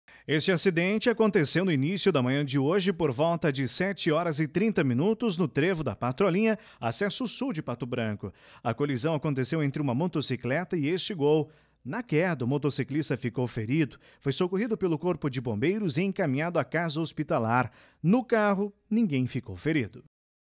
Acidente de trânsito deixou uma pessoa ferida na manhã de hoje (11)
Segurança: O acidente ocorreu na manhã de hoje (11) no Trevo da Patrolinha. A colisão envolveu uma motocicleta e um veículo de passeio.